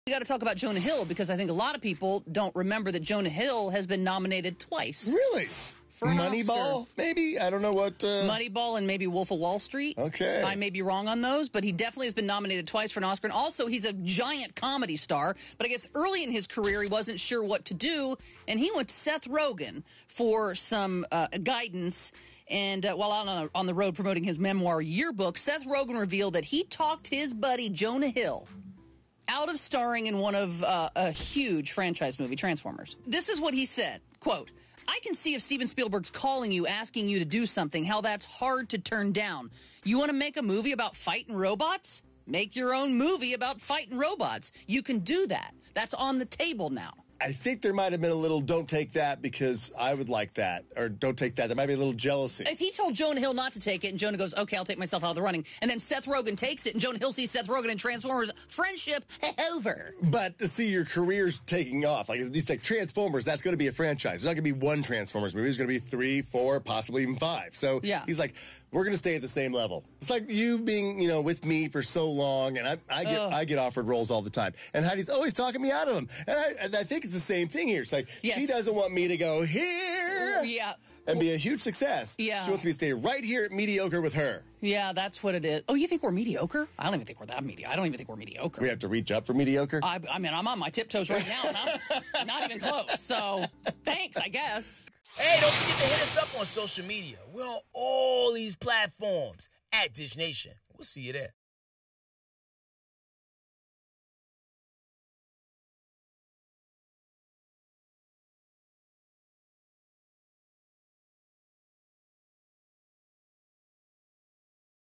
Seth Rogen Told Jonah Hill To Turn Down 'Transformers' And Make His Own Movie About Robots
SethRogen didn't want his friend JonahHill to transform his career! The actor persuaded Jonah Hill to turn down a role in a Transformers movie. Tune in to see what his alternative suggestion was and find out why Frank Kramer is questioning Seth's intentions.

HOSTS
ATLANTA: